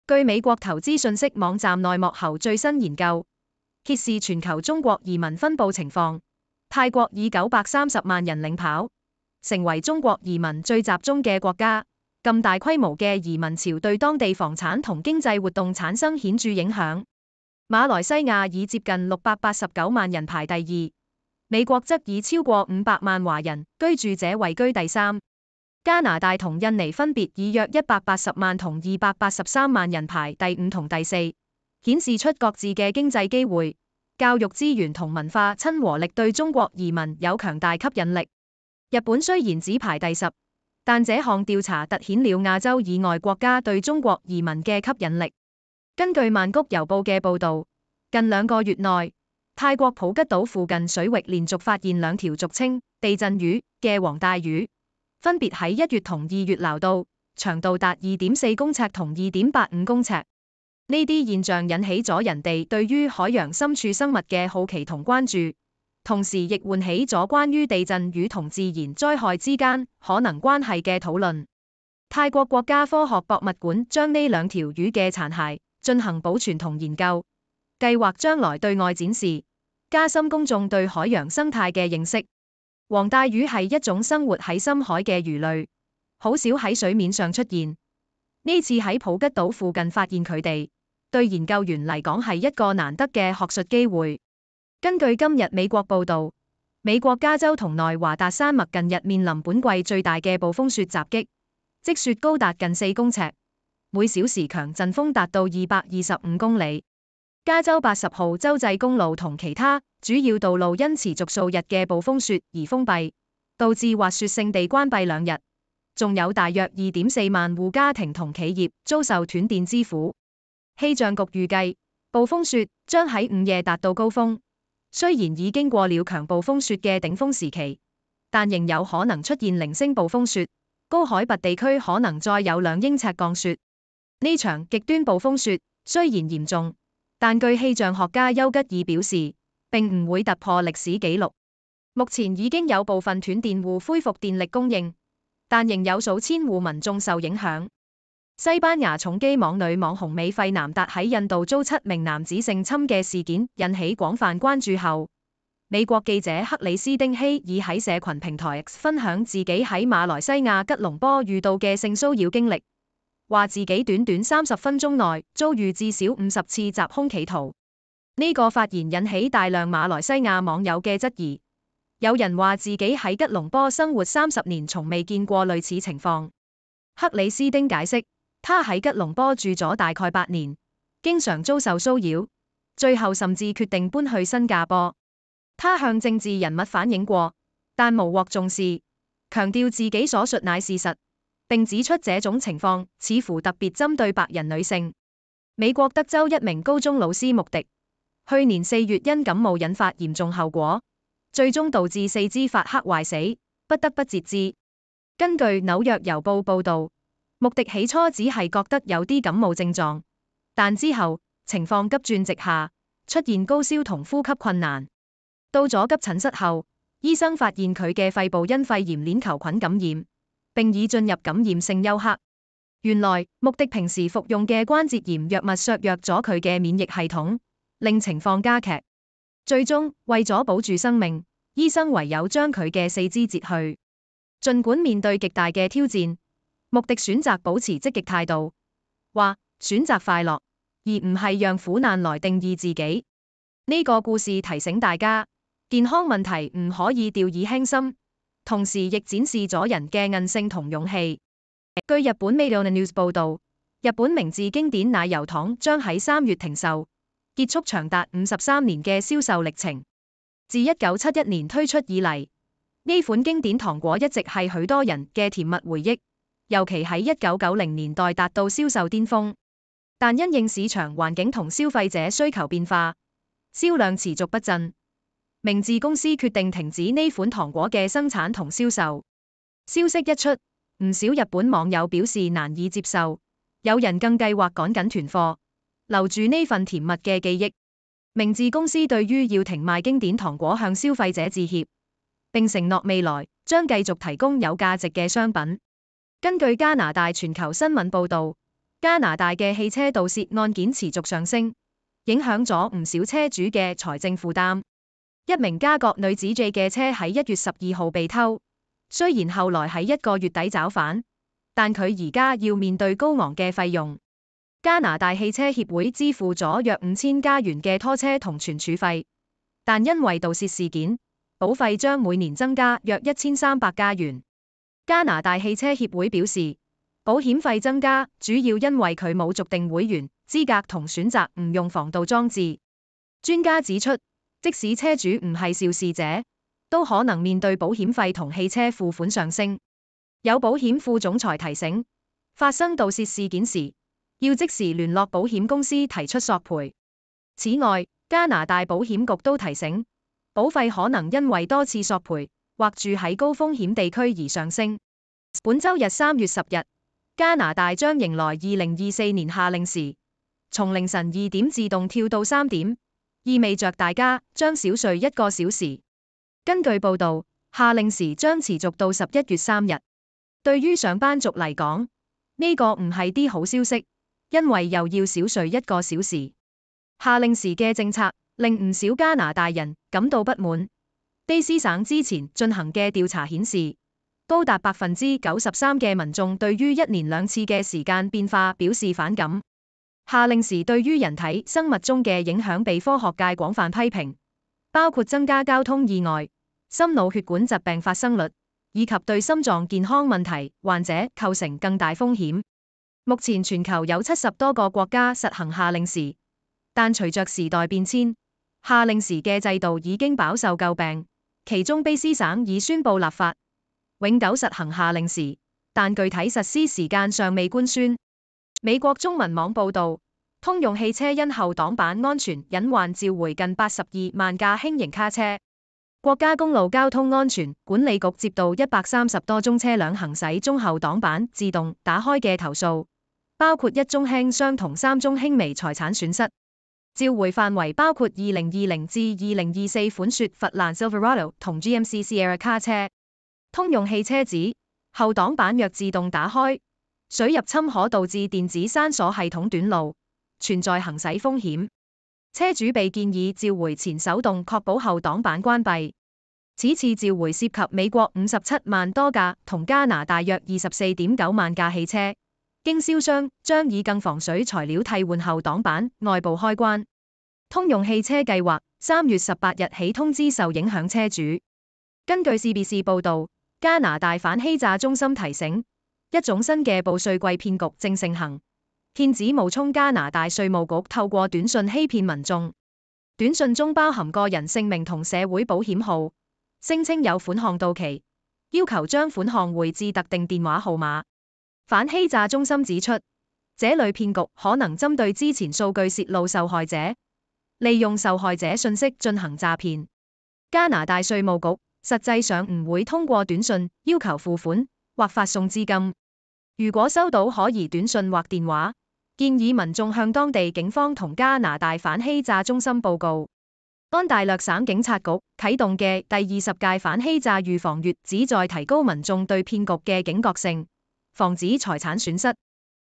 今日快訊粵語:最多中國人移民國家|泰國普吉島連續驚現地震魚|西班牙女網紅遭性侵美記者爆馬來西亞至少50次騷擾|日本明治經典糖果Chelse糖終章|加拿大報稅季新騙局謹防假CRA短信
泰國冠絕全球 最多中國人移民國家
泰國普吉島驚現「地震魚」 連續二月發現
美國加州暴風雪 狂風雪封山路斷電慘
西班牙女網紅印度遭性侵 美記者爆馬來西亞至少50次騷擾事件
美女老師敗血症截肢 鬥志未泯
日本明治經典糖果Chelse糖終章 銷售53載告終
加國盜車潮 車主保費飆升
加拿大夏令時周末轉鐘 鬥志喪失
GM通用汽车召回82萬輛輕型卡車 防後挡板隱患
加拿大報稅季新騙局 謹防假CRA短信